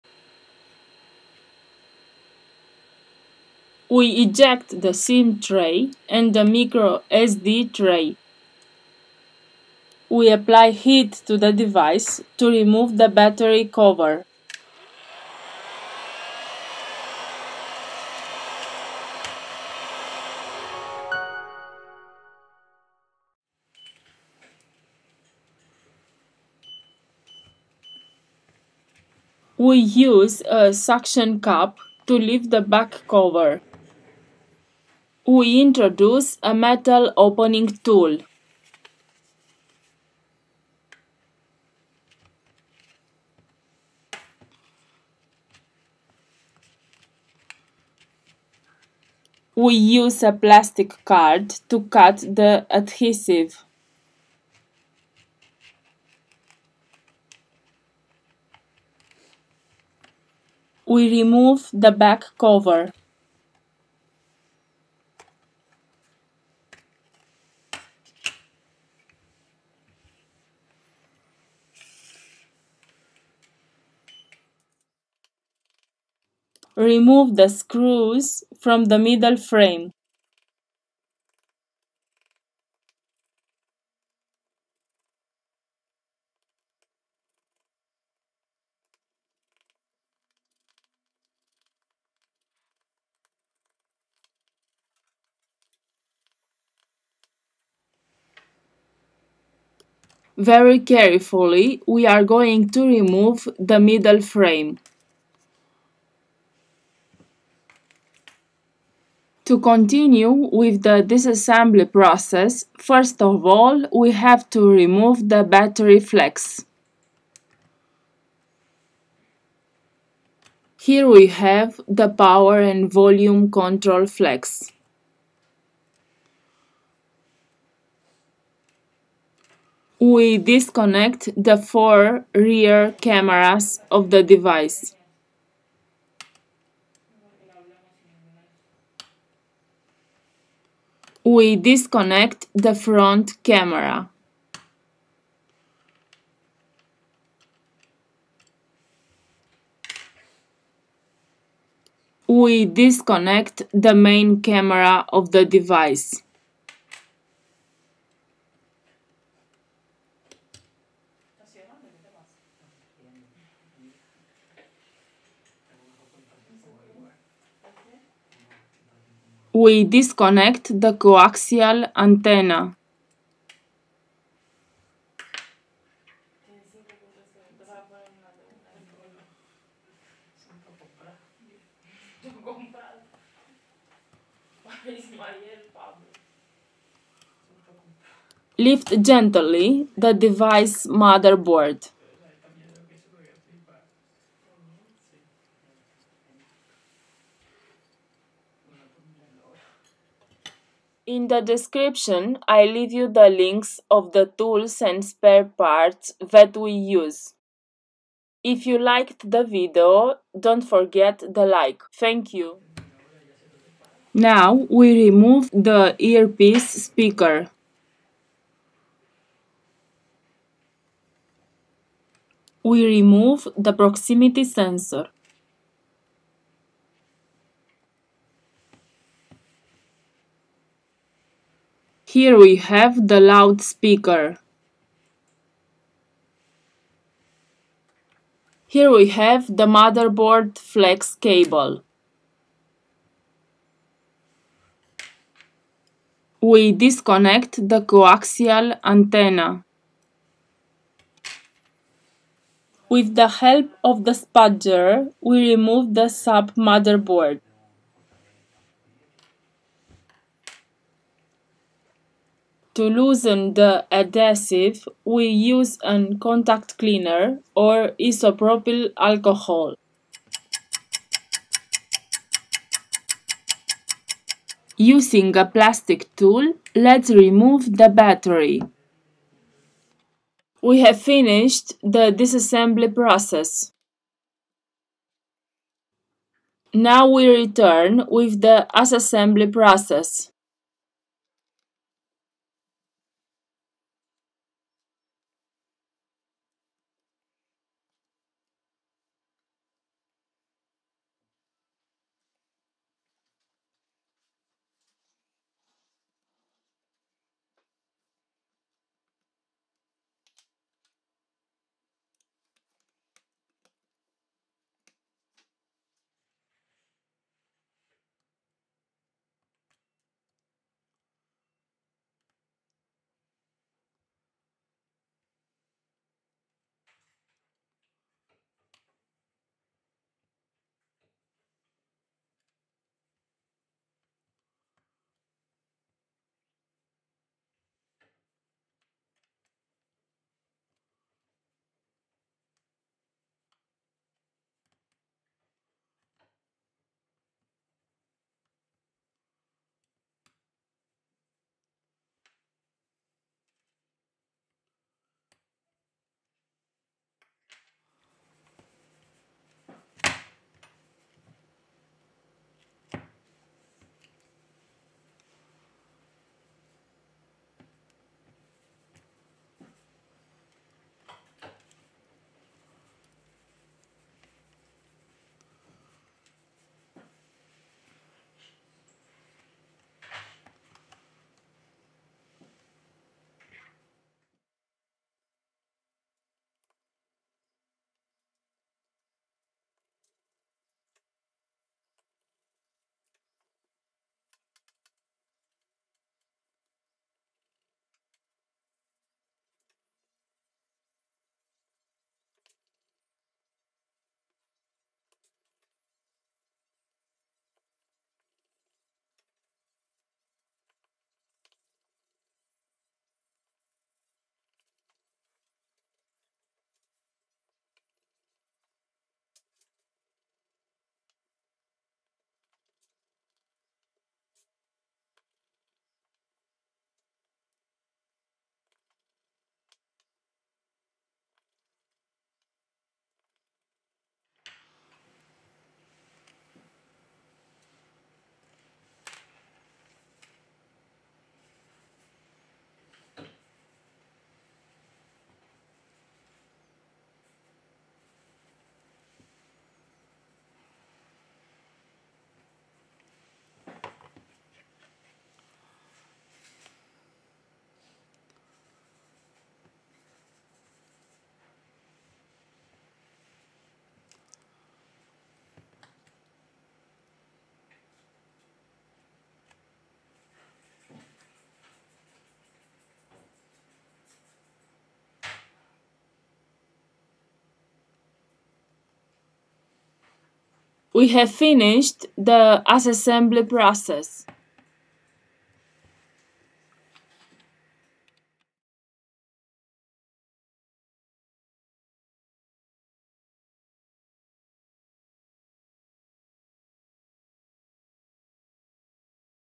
Huawei Nova 5T Teardown
Huawei Nova 5T full disassembly.

If you ask how to disassembly this device, Huawei Nova 5T  this video is what you are looking for because our technicians have had the opportunity to work this model in the workshops of MovilOne so, today we ll show the complete disarm of Huawei Nova 5T , so if you want and if you have the necessary tools you can repair any fault by yourself. Be careful and follow the steps that we are present below.